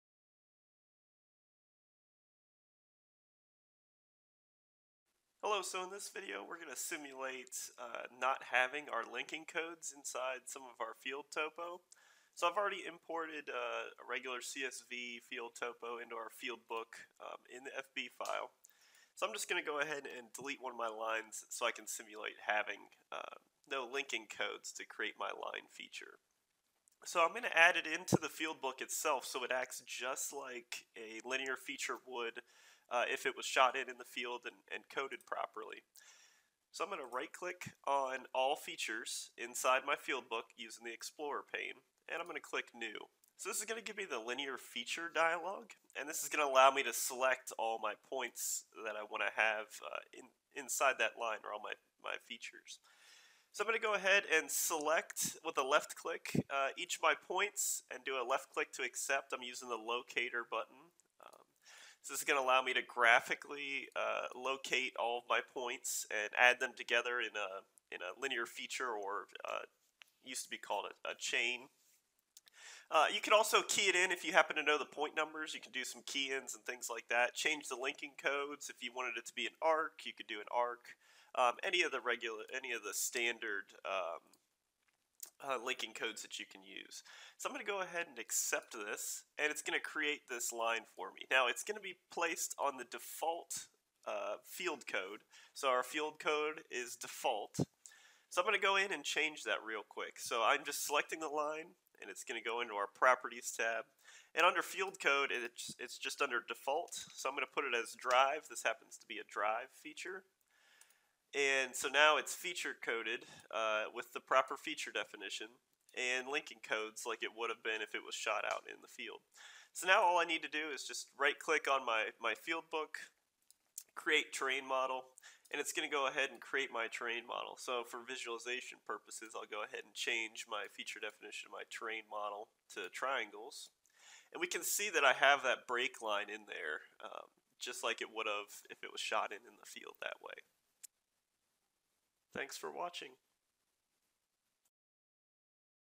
OpenRoads Survey Adding a Linear Feature to an Existing Field Book
This video discusses adding a linear feature into an existing Field Book with OpenRoads Designer.